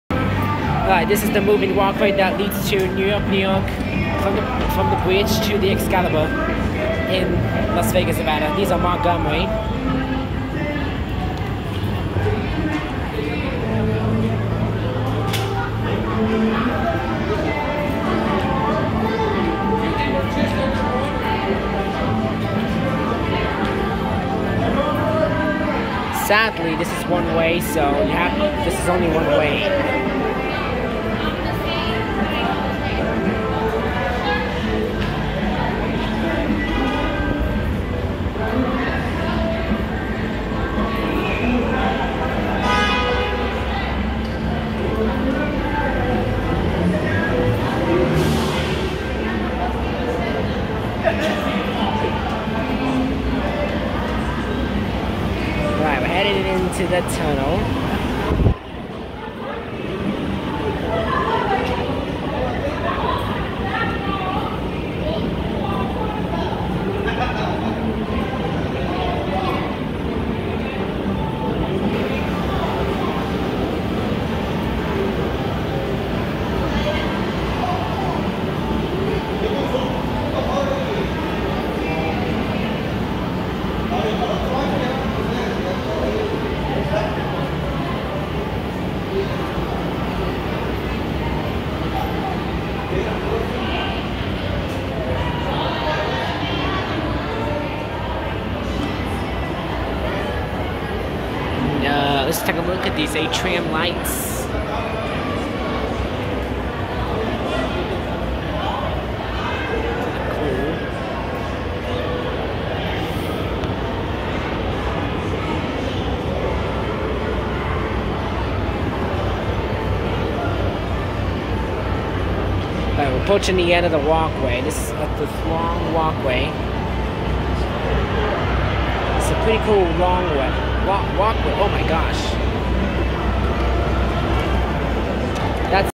Montgomery Moving Walkway From New York New York To Excalibur Las Vegas, NV (Season 9 Episode 8)
Escalator Info:

Brand: Montgomery
Model: ???
Step-Size: Double-File
Layout: N/A
Set Of Lights: None
Colored Steps: Gray
Shot: 9/3/22
Running Rating: 4.6